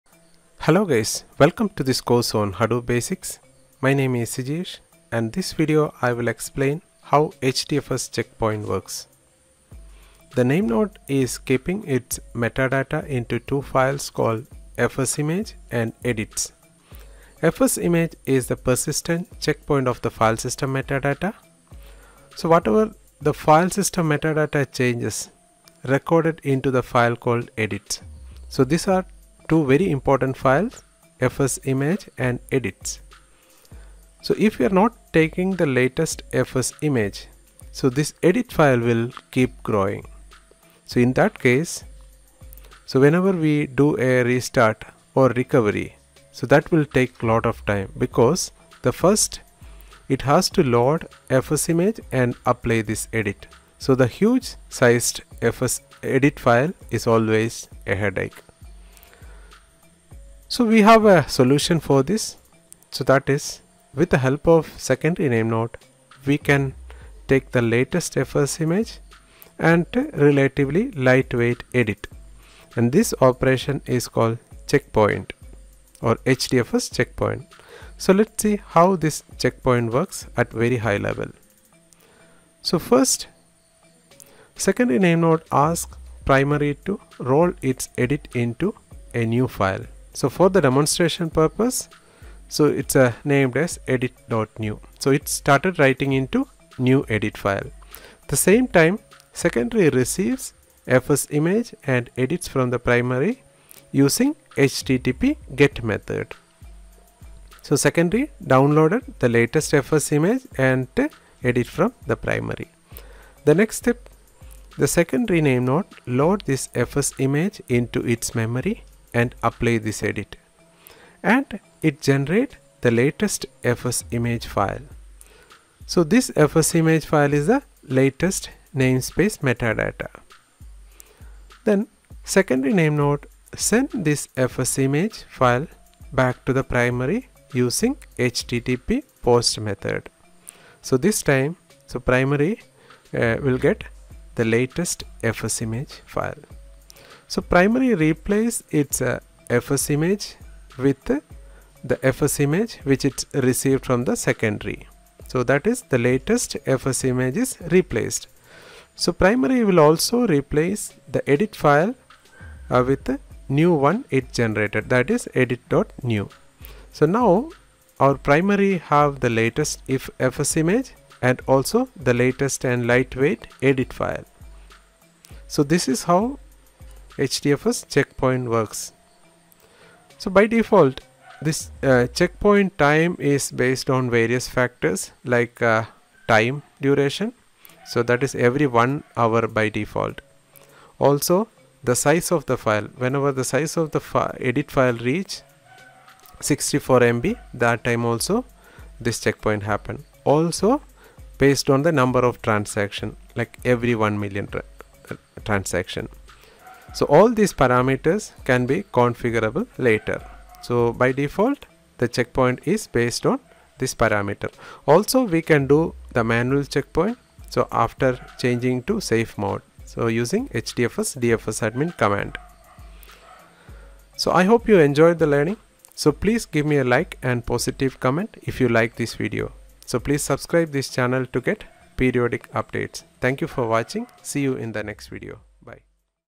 007 How HDFS CheckPoint Works
This video will explain how the HDFS check point works with the help of secondary namenode